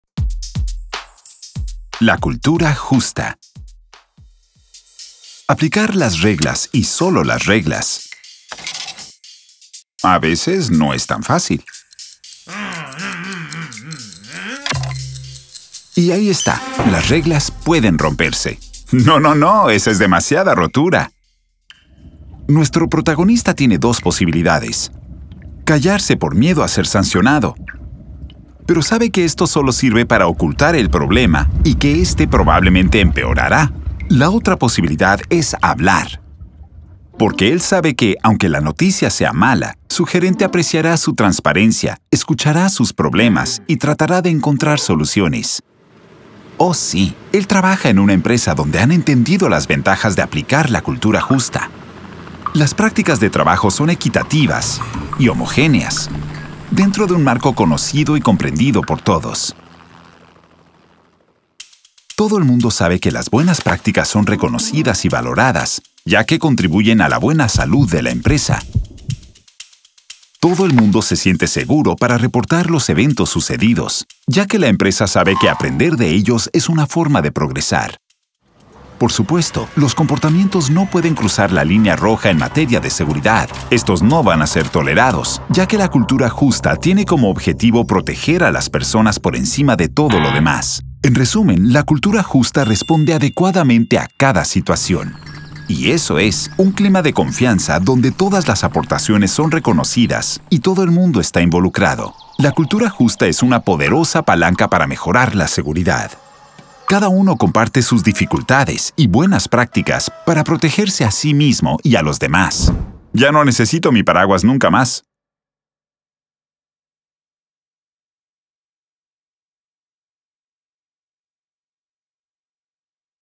La Cultura Justa
Nuestro héroe del día no va a desarrollar su tarea a la perfección…No, pero va a atreverse a hablar y de esta forma hacer escalar las informaciones para permitirle a la organización progresar, y si él no oculta su desvío es porque su organización ha desarrollado una Cultura Justa. Con acciones gerenciales equitativas, homogéneas, en un contexto comprendido y conocido por todos…¡los ingredientes indispensables para generar un clima de confianza!

Usted puede compartir y difundir este video a condición de citar al autor. No esta prohibida la modificación de este video